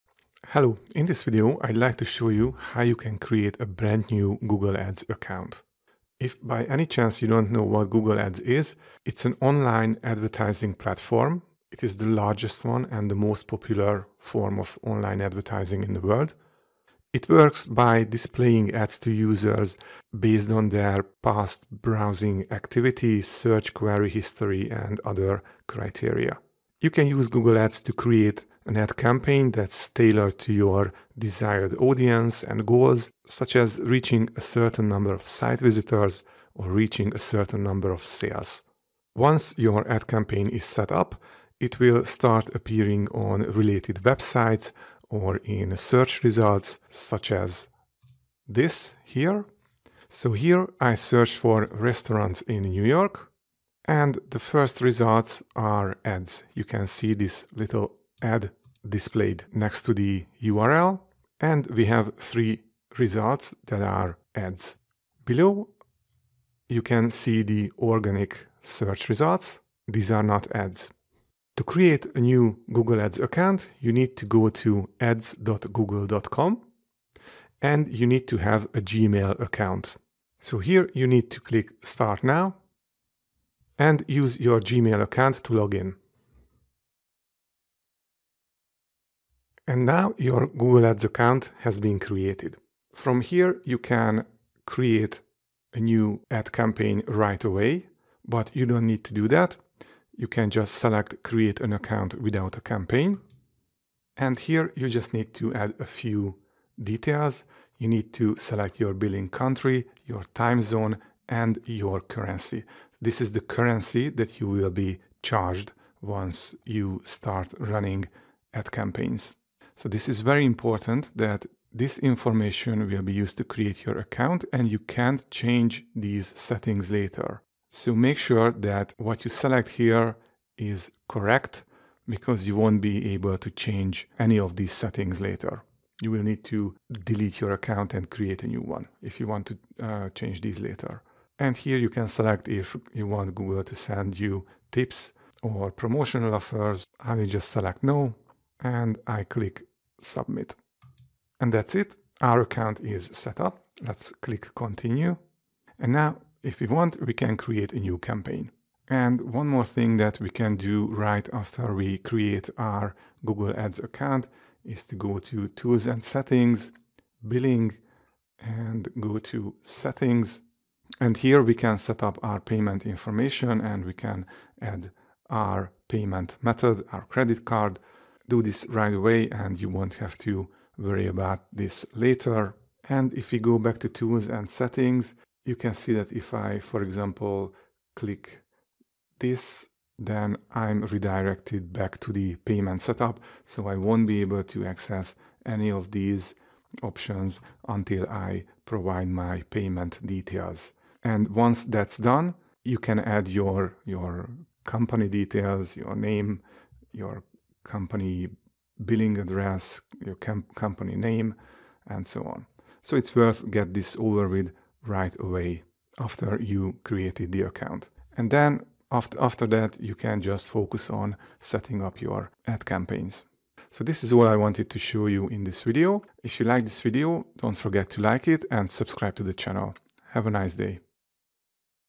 How To Create a Google Ads Account (2023)
In this video I will show you how to create a google ads account.

Google Ads is an online advertising platform developed by Google, where advertisers pay to display brief advertisements, service offerings, product listings, or videos to web users. It works by displaying ads to users based on their past browsing activity, search query history, and other criteria. You can use Google Ads to create an ad campaign that’s tailored to your desired audience and goals, such as reaching a certain number of site visitors or reaching a certain number of sales. Once your ad campaign is set up, it will start appearing on related websites, search results when someone searches for one of your targeted keywords. Each time someone clicks on your ad, you pay Google a certain amount of money.